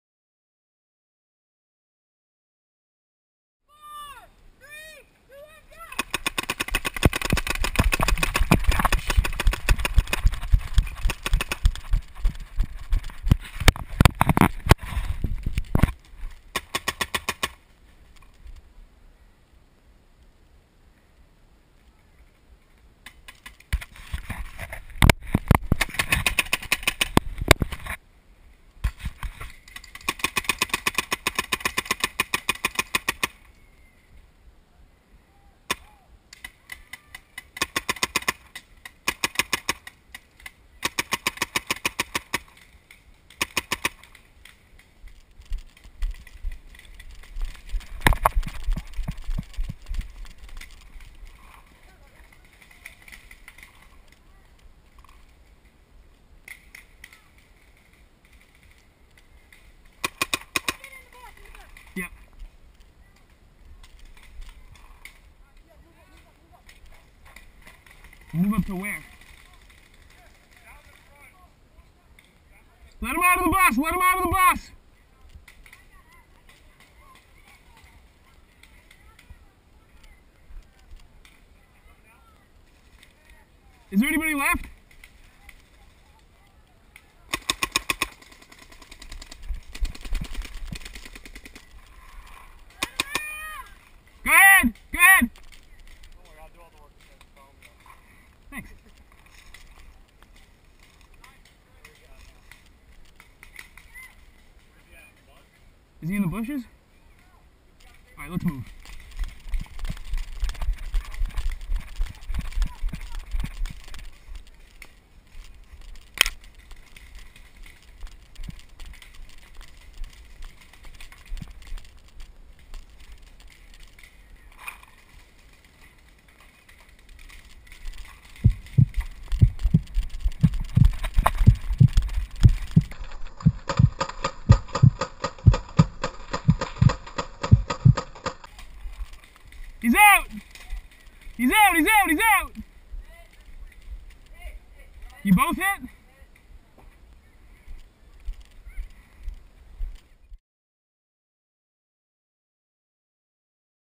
Hot Shots Paintball 2-10-2013 GoPro Action by Adam Parks
This is a short video we made while playing paintball at Hot Shots in Loxahatchee, FL in February 2013. Adam Parks from NSA Paintball and Branding Arc is wearing the camera was wearing a headband mount to capture the action.